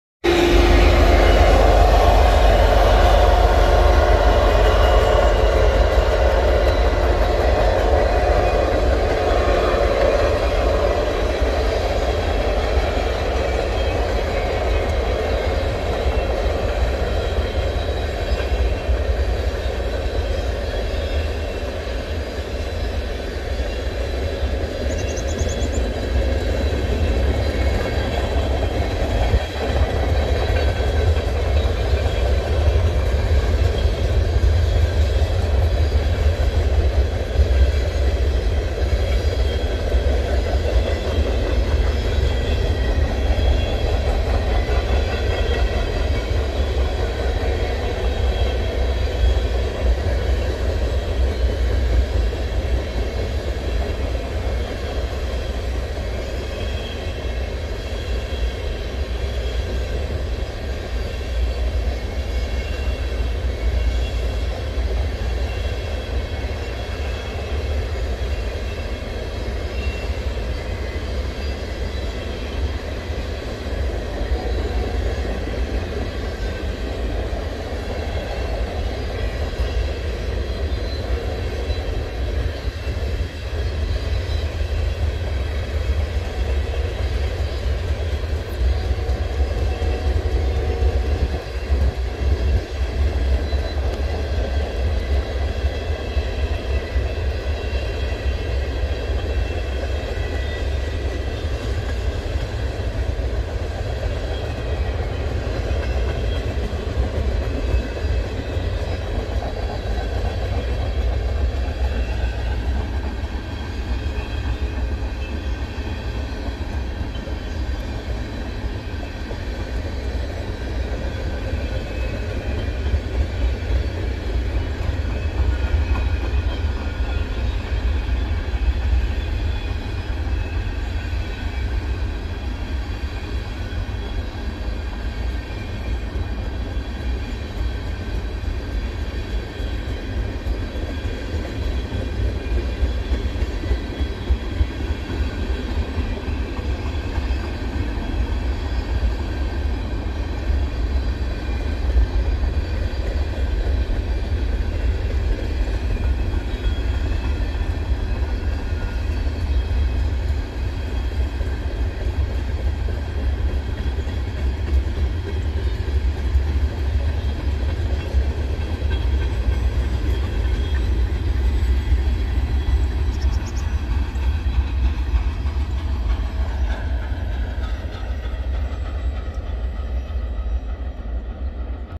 CN A492 w/bcol/gtw
1/25/23 12:51 This one snuck up on me. Normally, I'd just let it go, but when I stood up and looked out the front window to see what it was and saw the power, I couldn't let it slip by. Sorry, it's a little shaky... 432 axles, 26⁰, 27mph